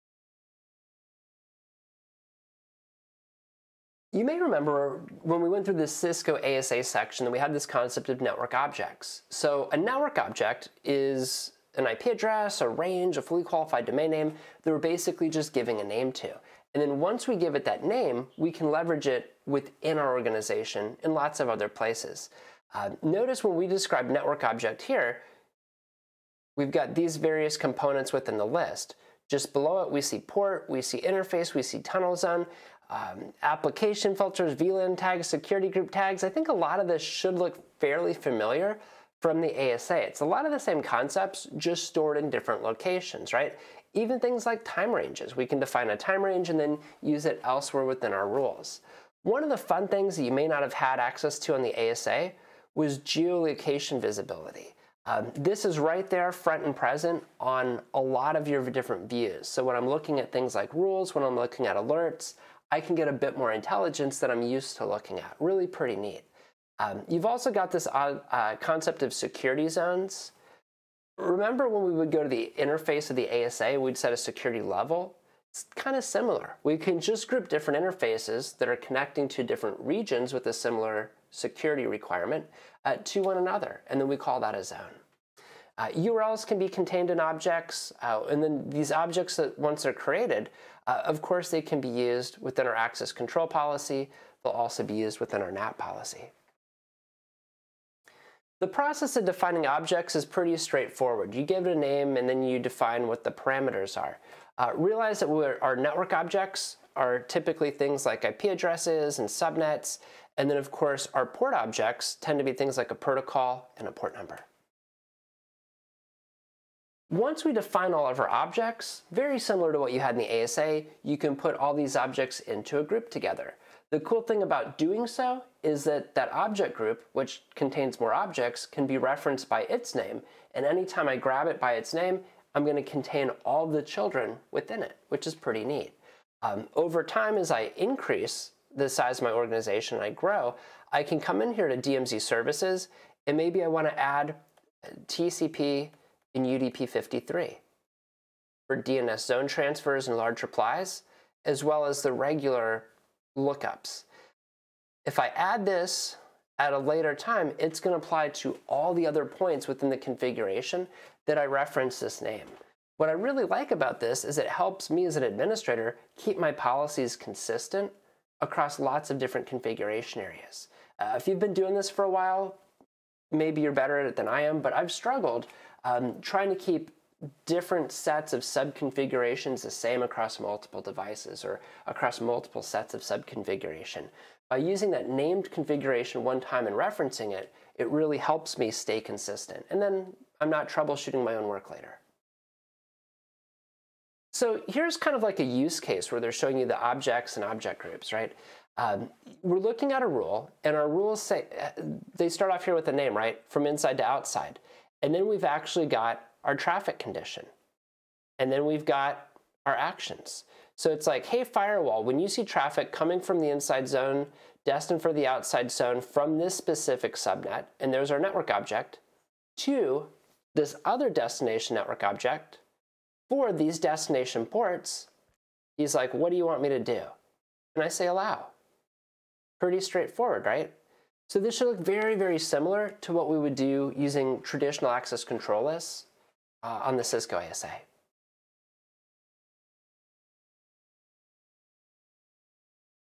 Cisco Firepower NGFW Objects - Part3
SCOR Cisco Training: Part 7 Deploying a Cisco Firepower Next Generation Firewall

In this module you will learn:

Cisco Firepower NGFW Objects,
Defining Object group,
Objects and Object groups Use Cases